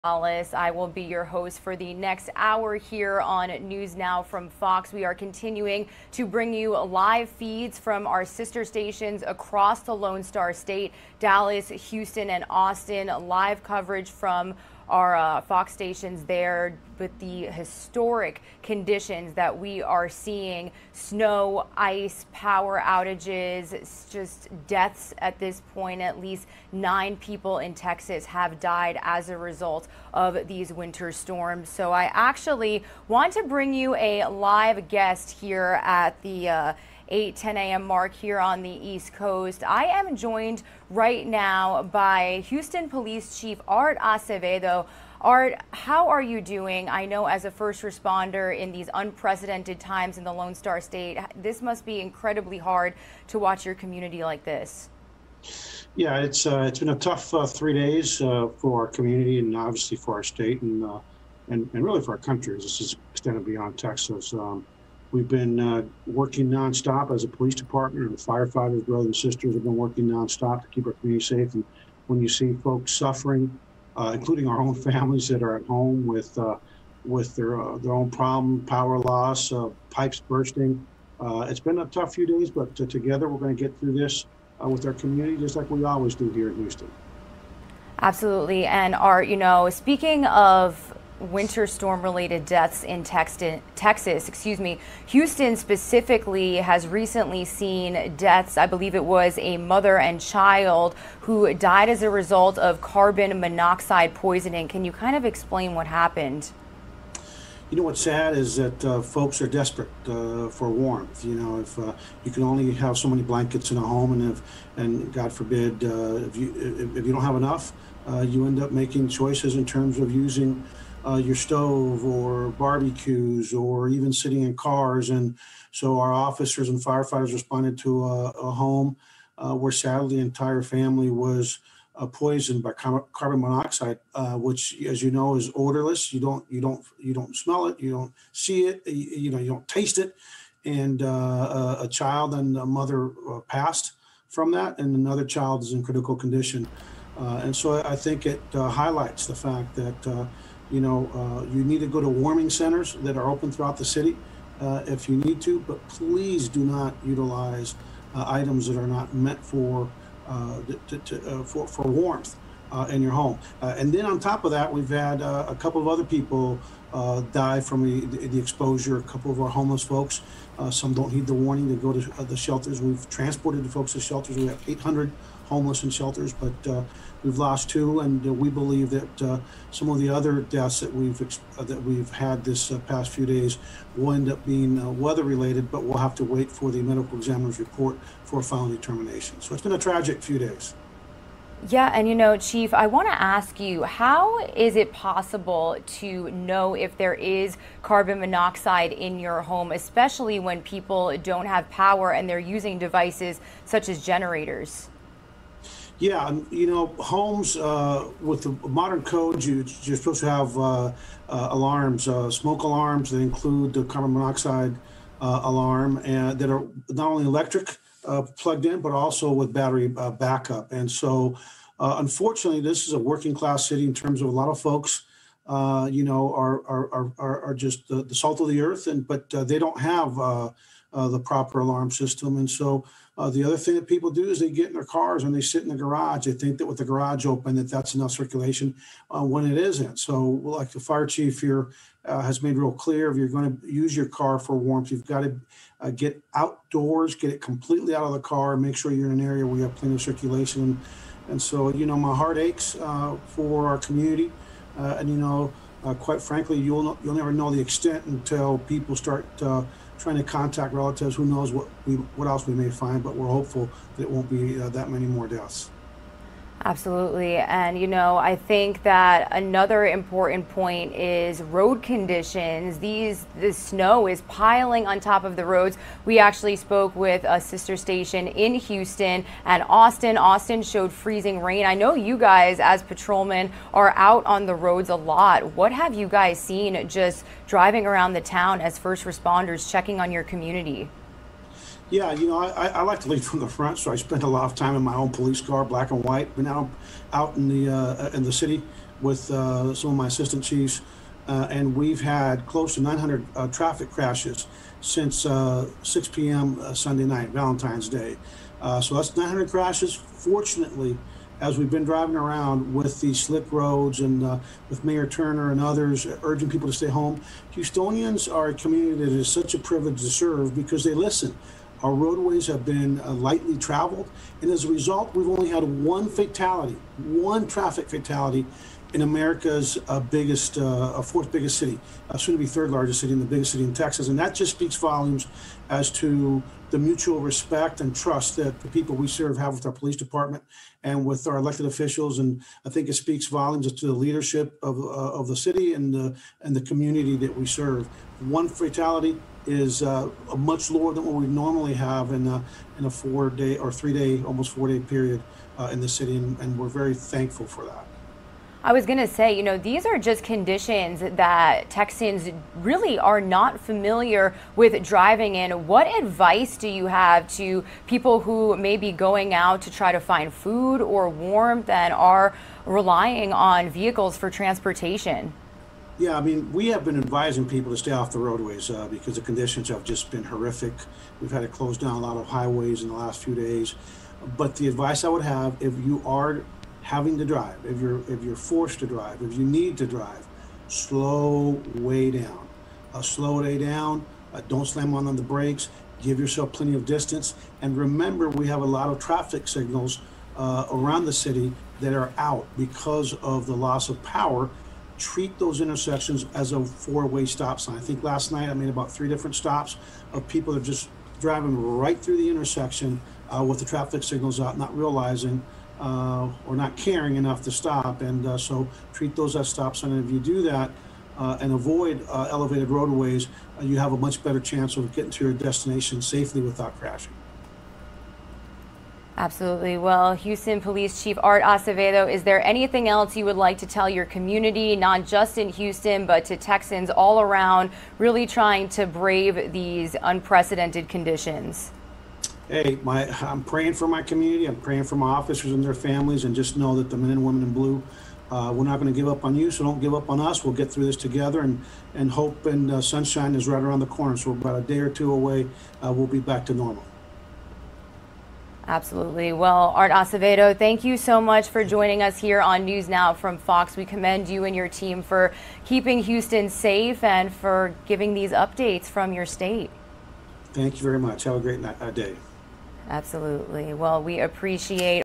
Carbon Monoxide related deaths, Illnesses as Houstonians try to keep warm | NewsNOW from FOX
Frigid, unusual winter temperatures and power outages are causing Texans to search for alternative ways for warmth. As Houston Police Chief Art Acevedo explains to NewsNOW from FOX's Regina Gonzalez ... that can turn deadly.